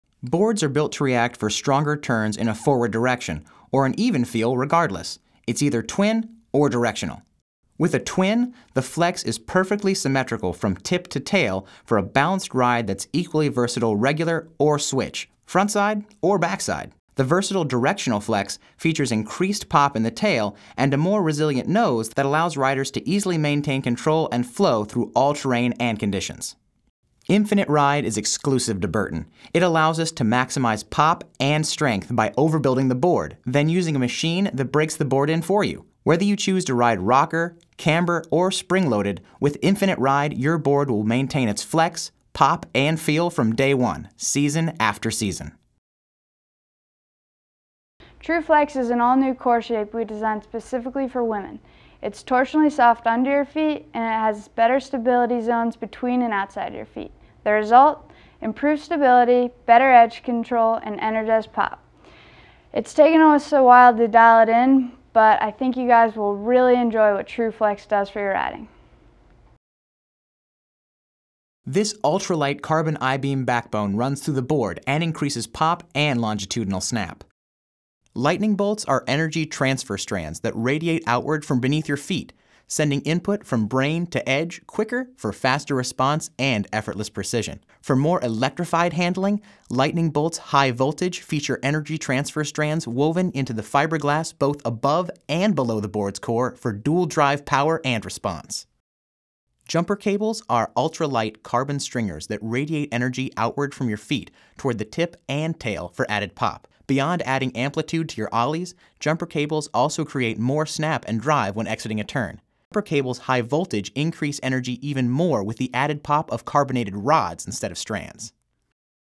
Explaining the Different Burton Flex Patterns
Video explaining the different flexes offered in the 2011 Burton snowboard line up.